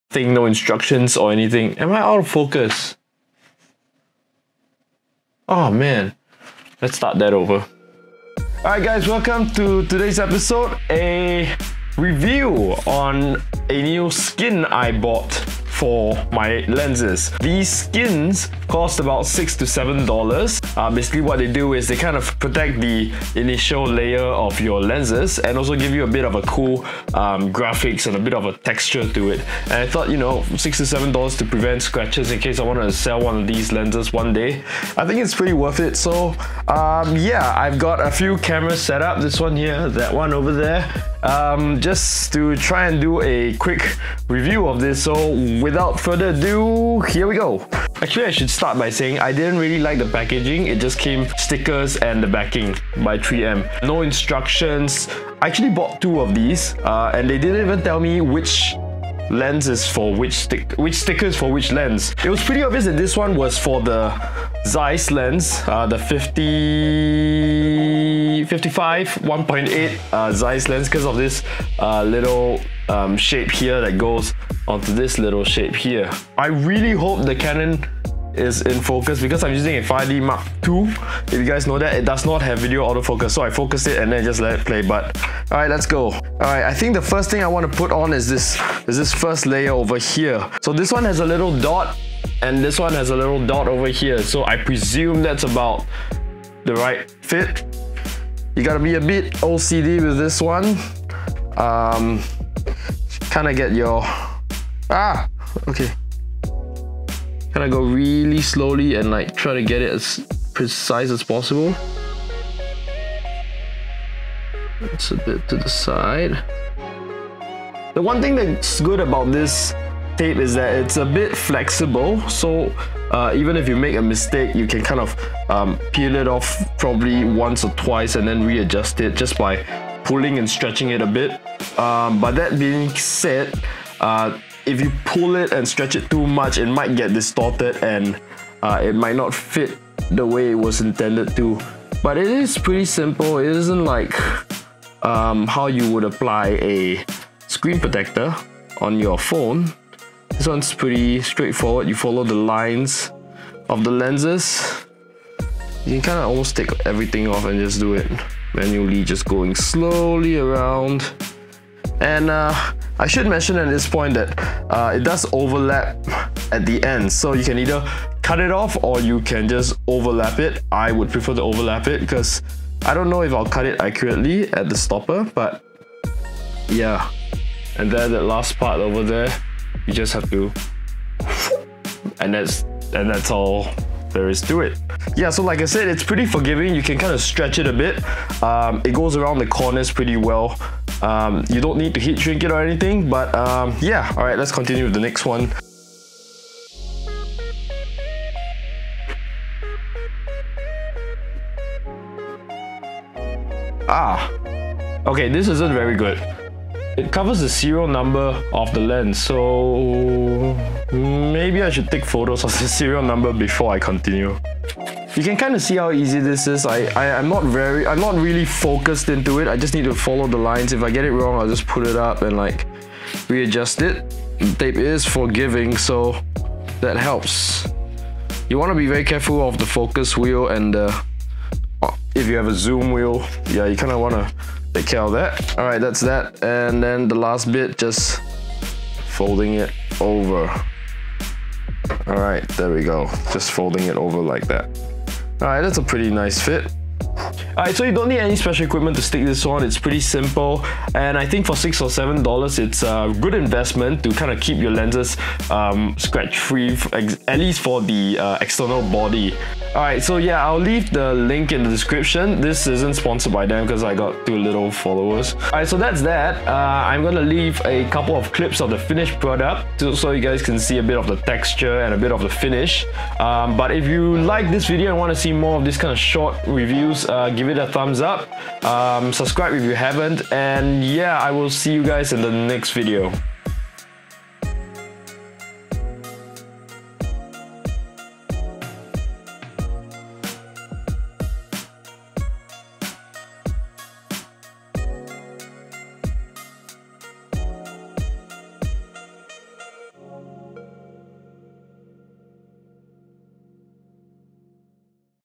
Lens Guard Camo-skin Review
Theses skins are inexpensive and there are many other brands out there that sell other designs  and textures. I bough these for USD $7 each and have no regrets. A simple way to protect your lenses against scratches to the external body.

~ Gear ~

MIC - Rode Video Mic Pro Plus
MONITOR - SmallHD Focus 5